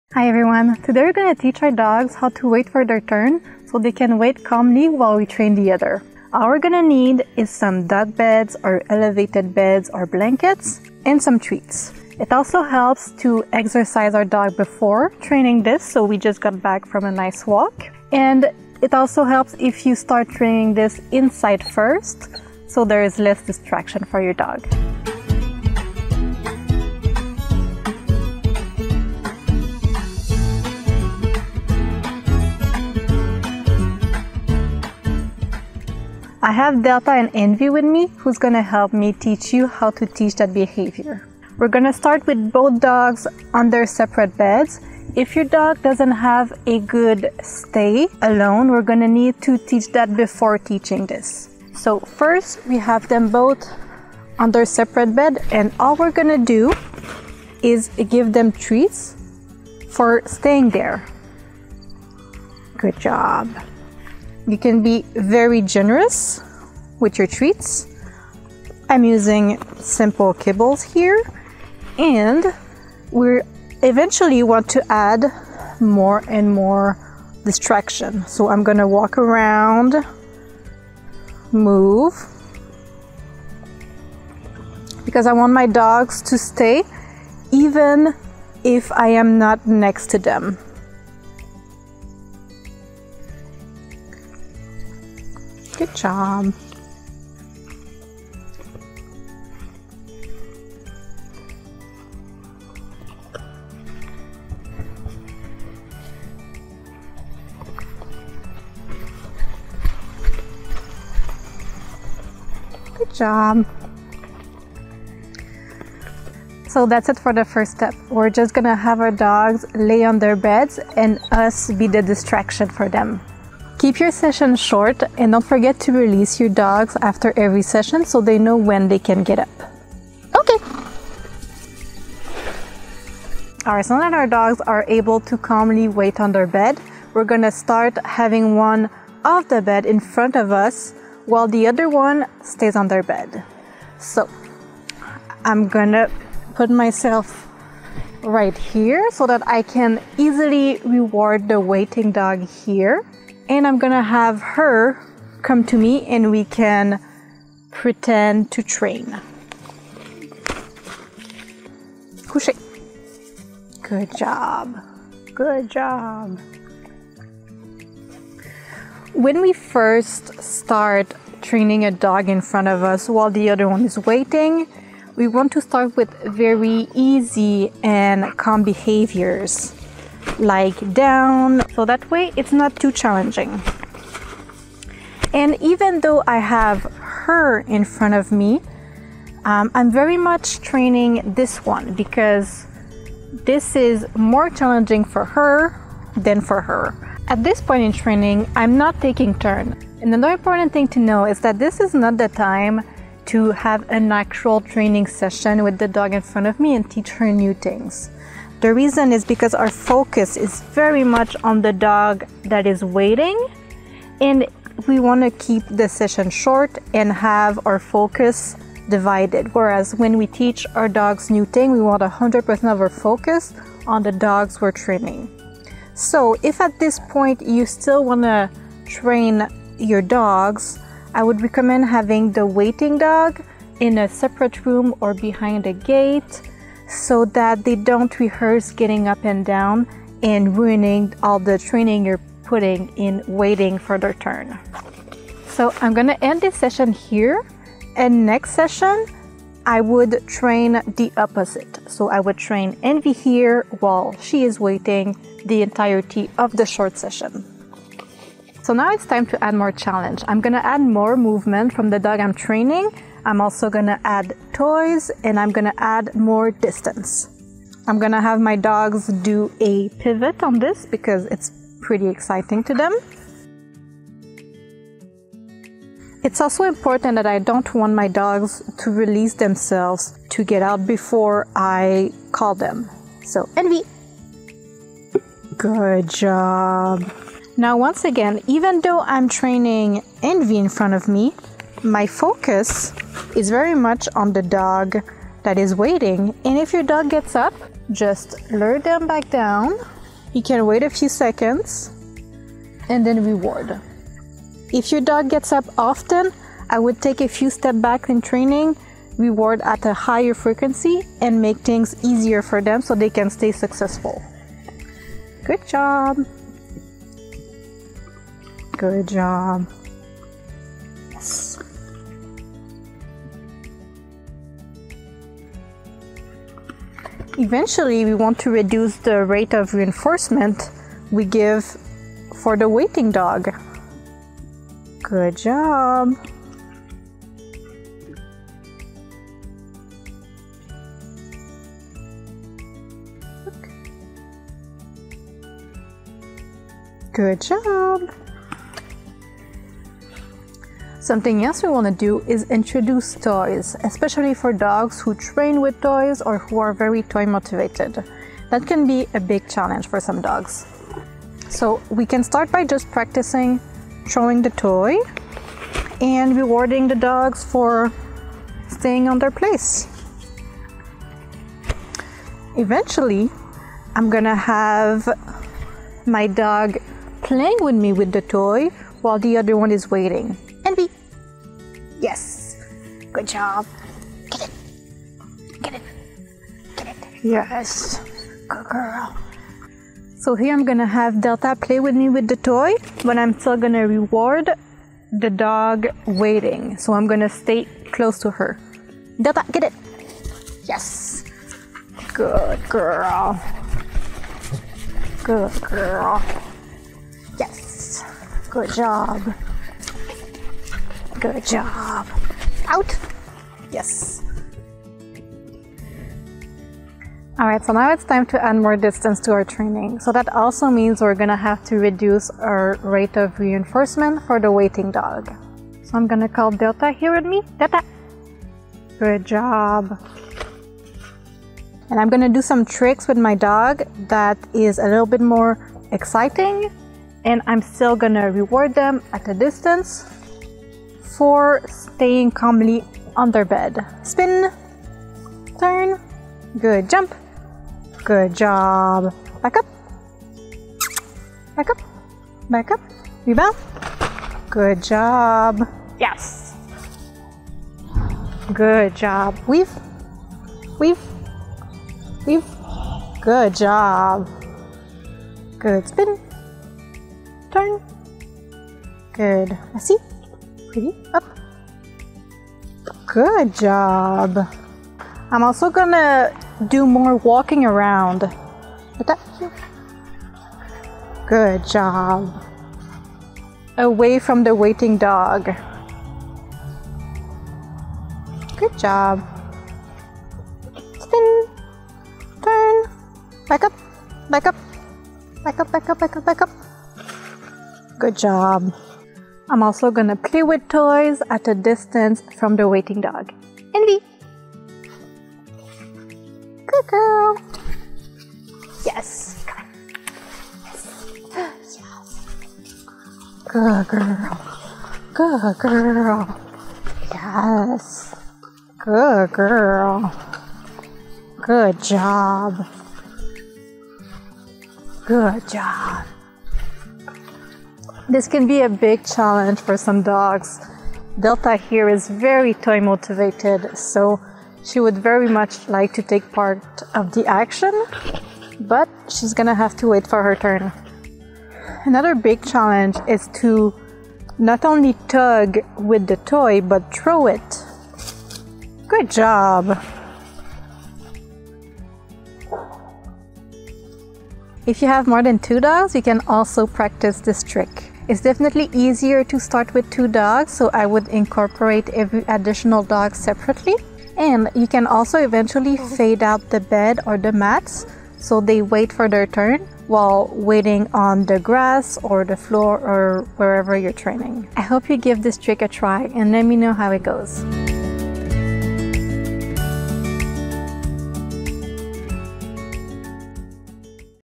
Teach Your Dogs To Wait Their Turn
When you have two or more dogs, it is crucial that they can wait their turn during training! Nothing is more frustrating than having one dog constantly interfere.

It not only disrupts your training flow ... but can even lead to resource guarding and escalations!
So let's teach your dogs how to wait their turn patiently and independently :)

If you only have one dog, this is still a valuable exercise to do. There will be times when you need your dog to be patient and not interfere (eg during cooking, while doing housework, entertaining guests, working from home etc.)

Invest time in teaching this kind of impulse control - you won't regret it!

00:00 Introduction
00:38 Step 1: Calm Sitting
02:14 Step 2: One Dog Training
04:19 Step 3: Adding Movement
05:51 Step 4: Adding Toys
06:58 Step 5: Increasing Distance
09:15 Step 6: Adding More Dogs